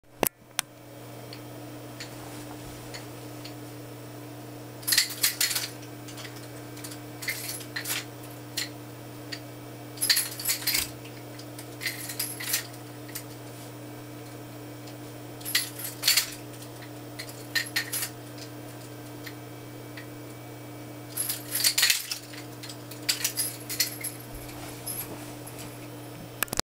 Recovery Aerial [Freechaku] ~ SSFNA ~ WDS
When you end up holding the chain, out of choice or not, here's a simple way to regain control and get back to holding a bar. Kill the momentum then you can almost always flick them in to an aerial on a predictable path to catch one or both bars ~